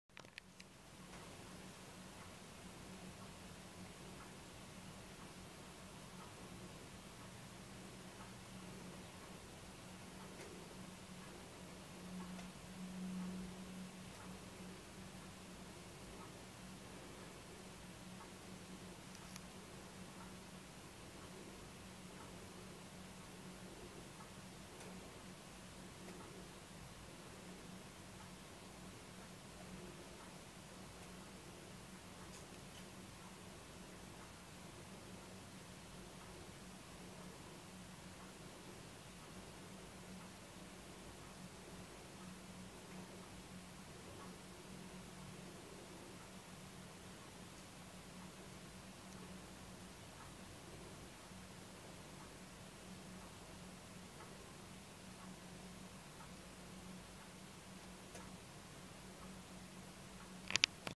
Wood pigeon in the rain
Woody making the most of a downpour to take a shower and scrub up